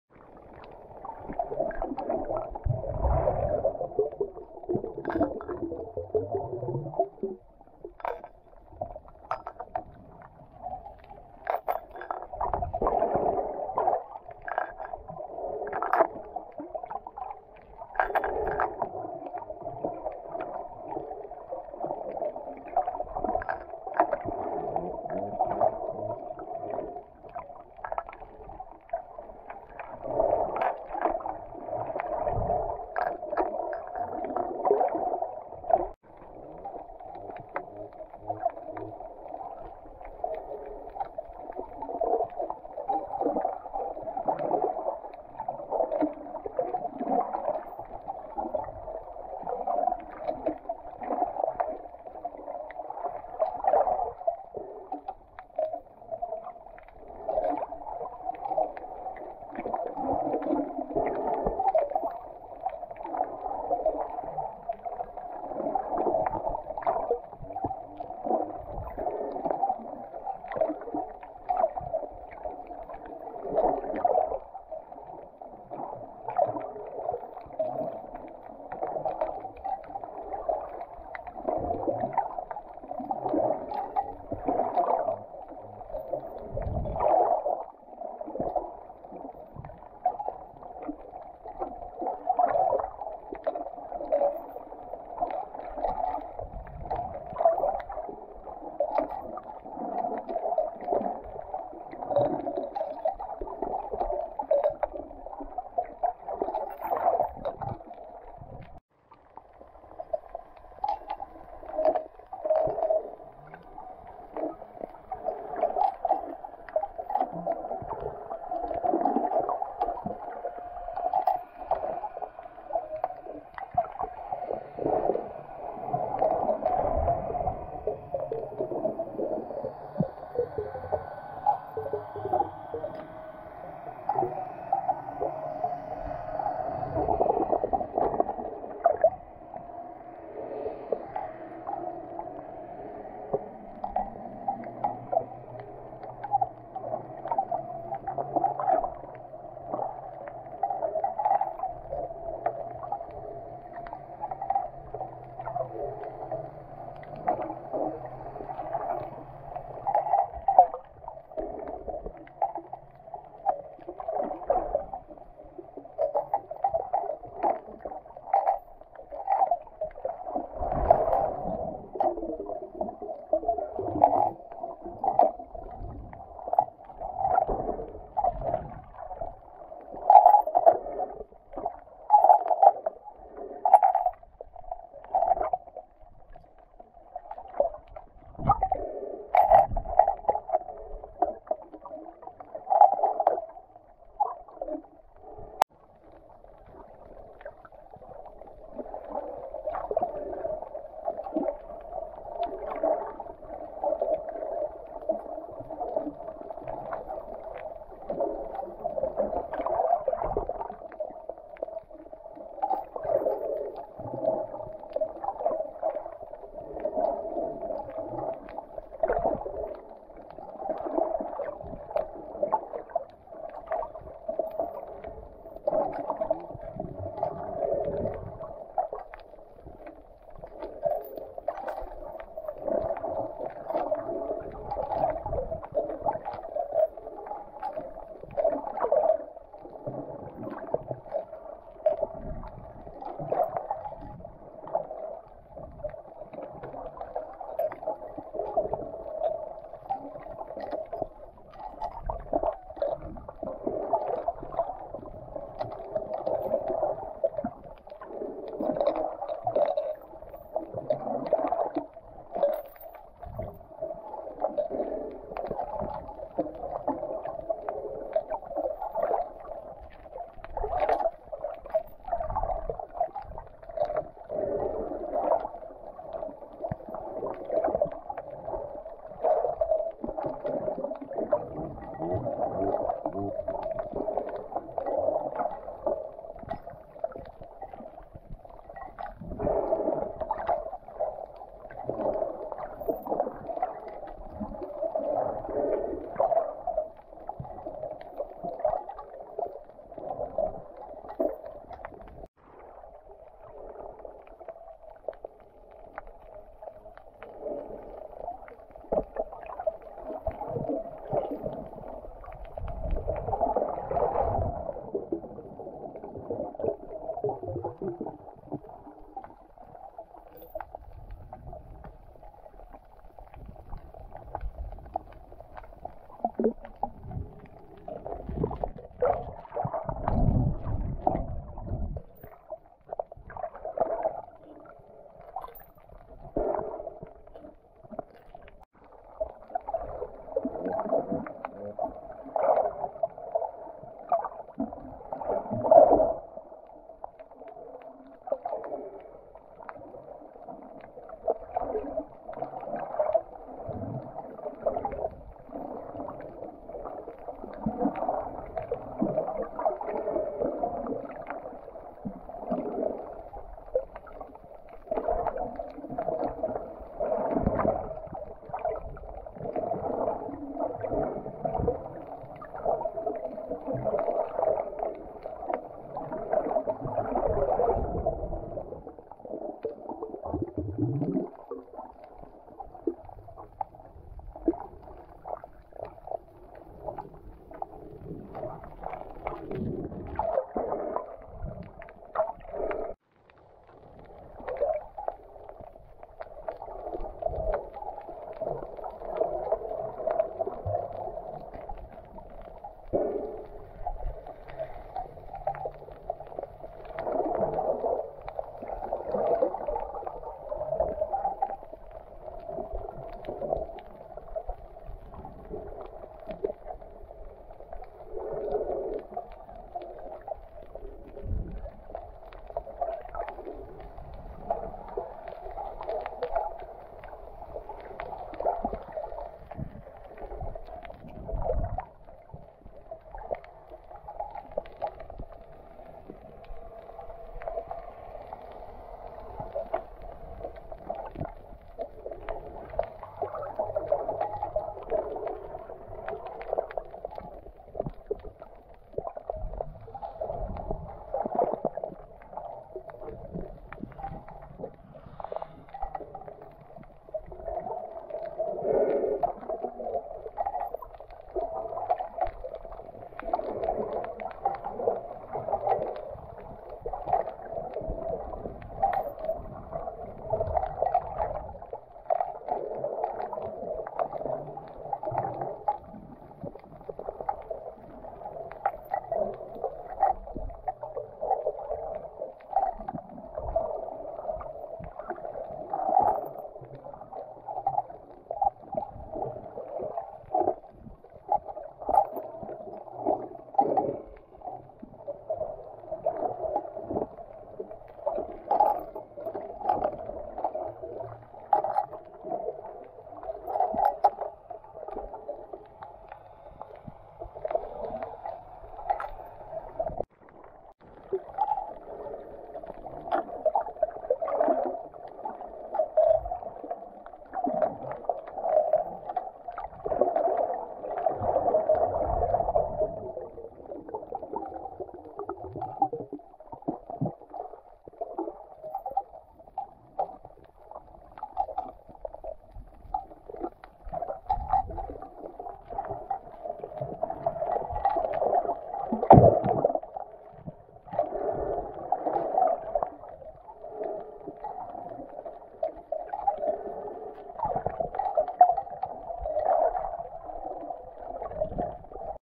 A Little Stroll at Turneffe Atoll
From my visit to Turneffe Atoll Marine Reserve: hiking the underwater trail.